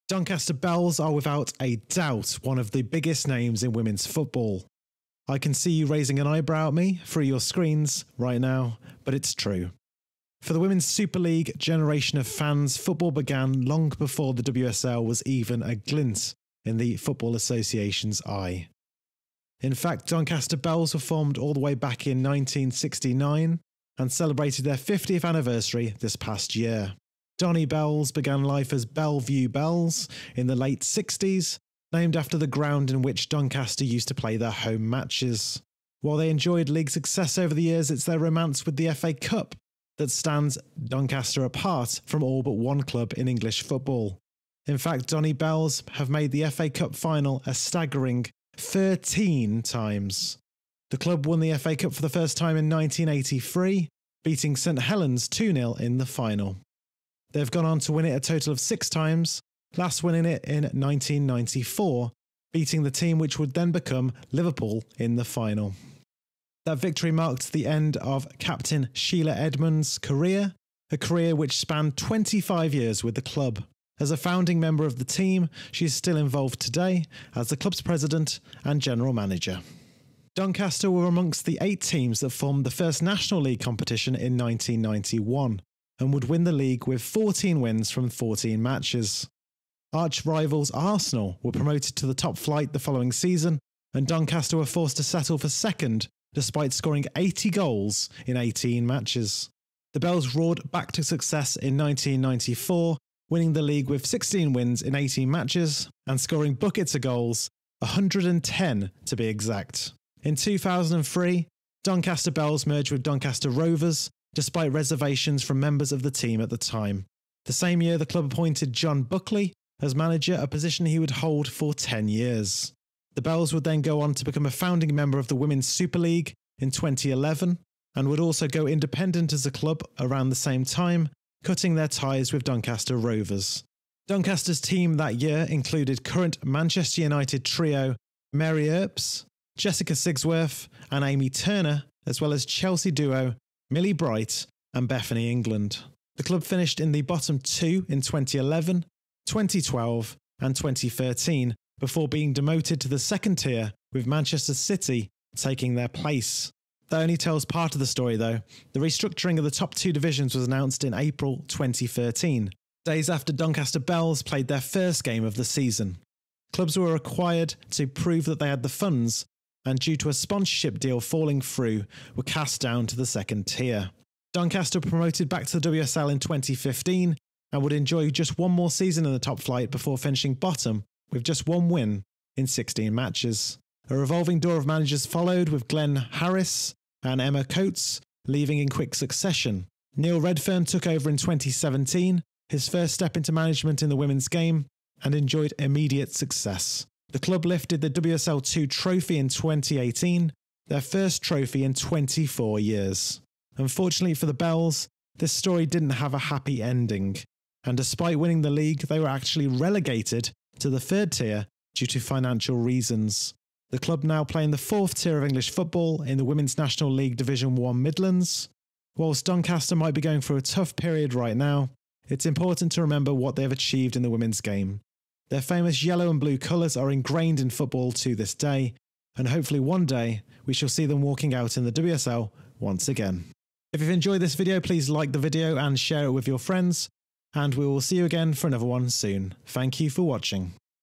The Rise & Fall of Doncaster Belles - Women's Football History
Doncaster Belles have been a big part of women's football since their inception in 1969.

A team that was once battling it out with Arsenal for national dominance, the Yorkshire side now play in the fourth tier of English football.

What happened to the six-time FA Cup winners? We look back at the recent history of Donny Belles and some of the great players that have graced the famous yellow and blue shirts.

You can learn more about them by watching Natalie Higgins' short documentary on the team